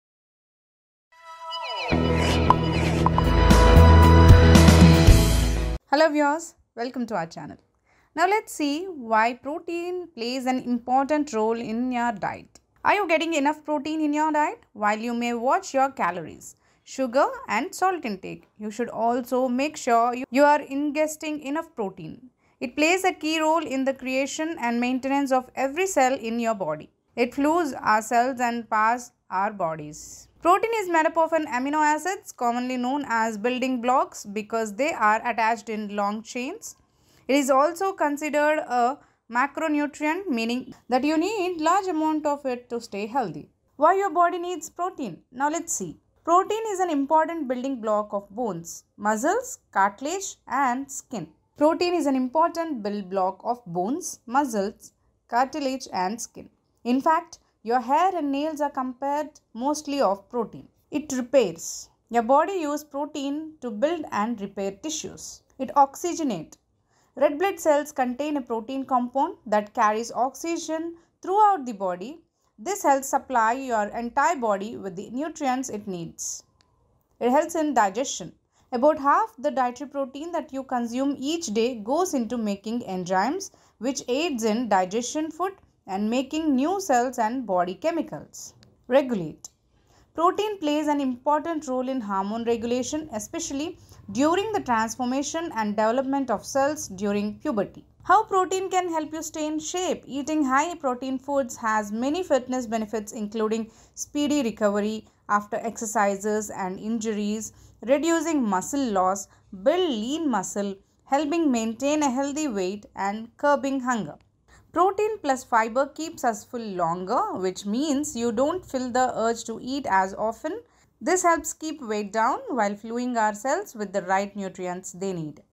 What Happens To Your Body When You are on High Protein Diet | Orange Health
Protein has become a buzzword when it comes to dieting and working out, but what does a high protein diet really do for your body?